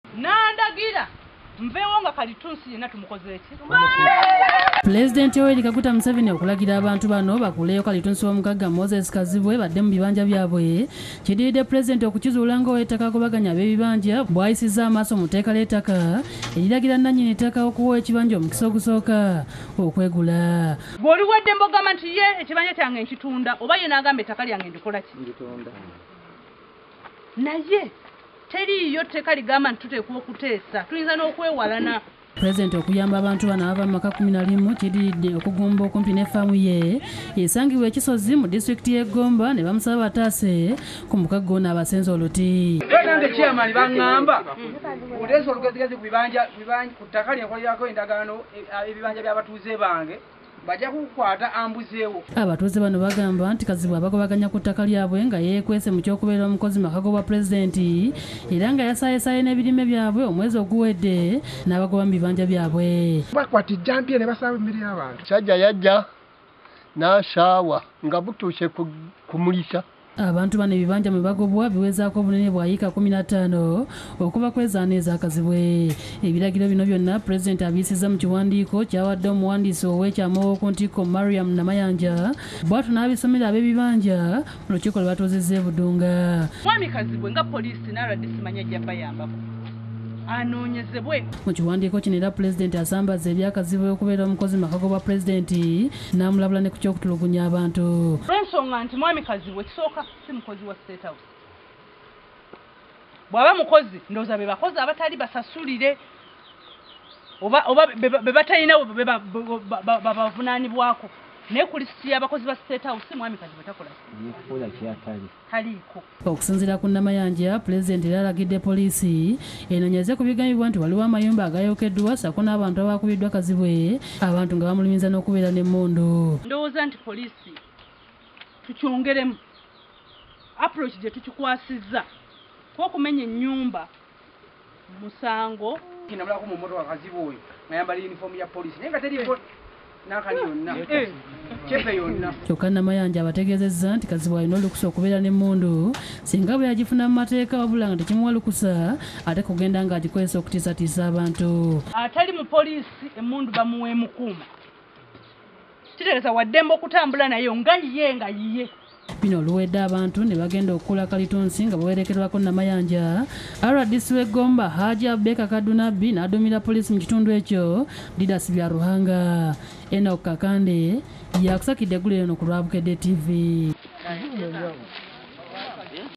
Pulezidenti azzizza abatuuze mu bibanja
Abalagidde okukuula kalittunsi eyasimbiddwako